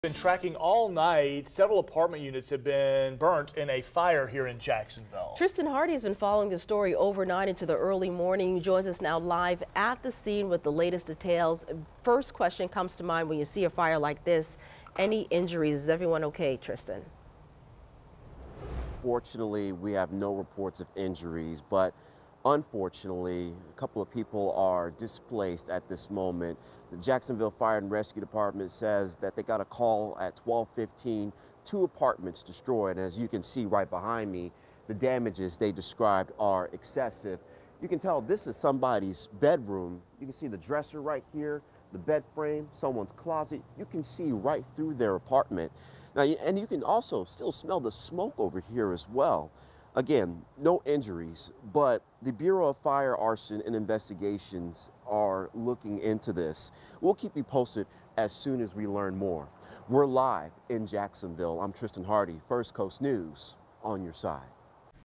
Report: Apartment fire on Jacksonville's Westside under investigation
No injuries were reported from the fire that happened at Westland Park Apartments on Collins Road in Jacksonville Wednesday morning.